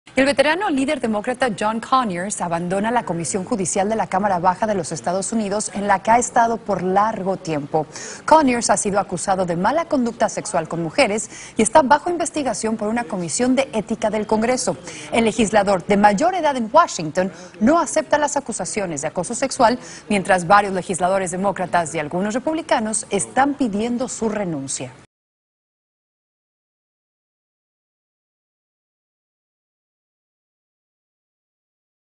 John Conyers anuncia que dejará comisión del Congreso tras acusaciones de acoso sexual
El veterano líder demócrata publicó en un comunicado que abandonará la Comisión Judicial de la Cámara de Representantes. "Le he notificado al liderazgo del partido mi intención de hacerme a un lado mientras se investigan estos asuntos", escribió.